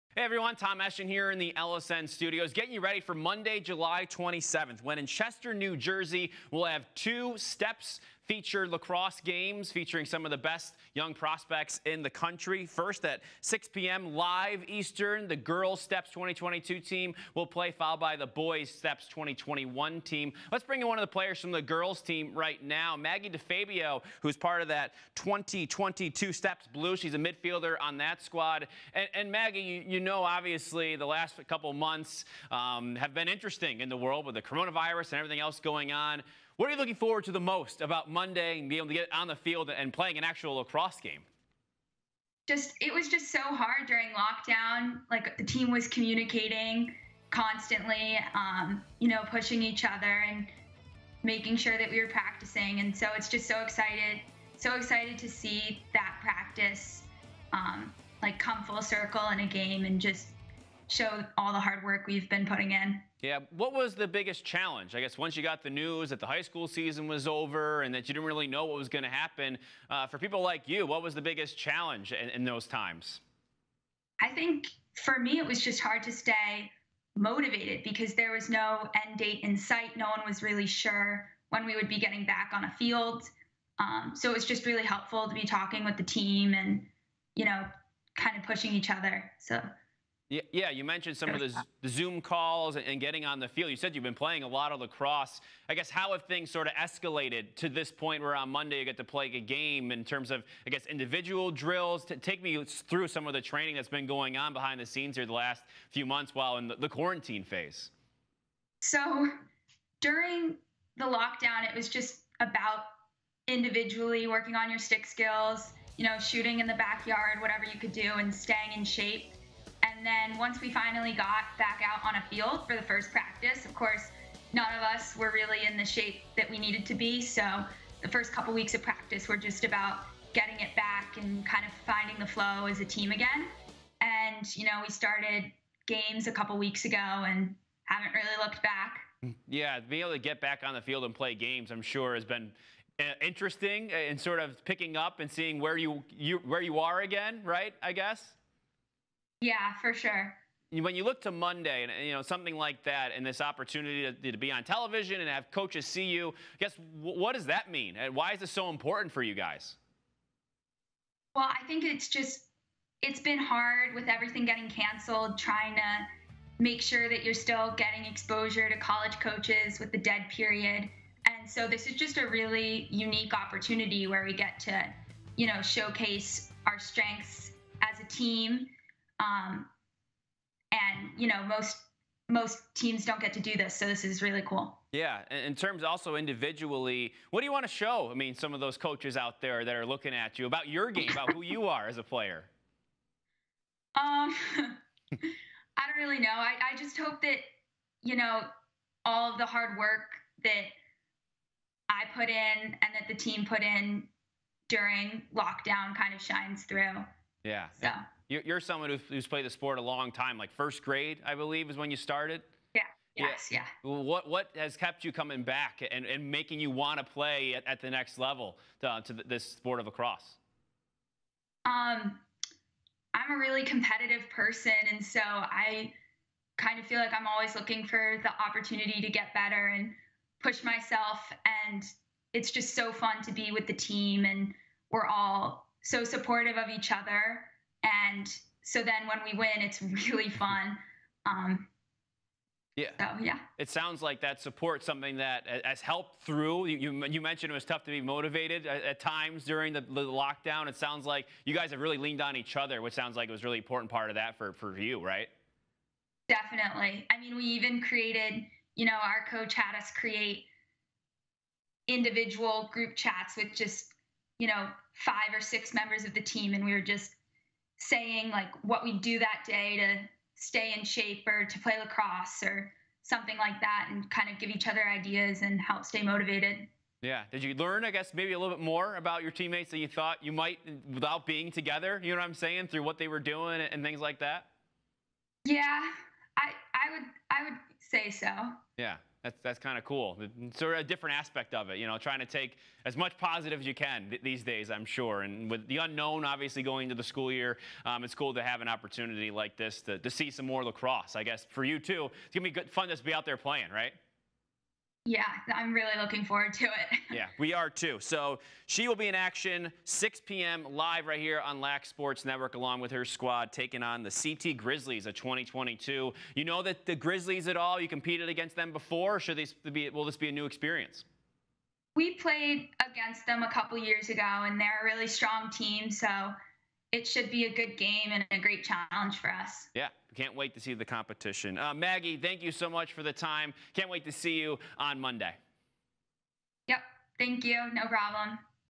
Maggie DeFabio, 2022 Midfielder talks to LSN about playing in STEPS showcase game
Maggie DeFabio from the 2022 STEPS women's lacrosse team talks about the upcoming game, what it means to the team, and how she has kept up her skills during the COVID-19 pandemic.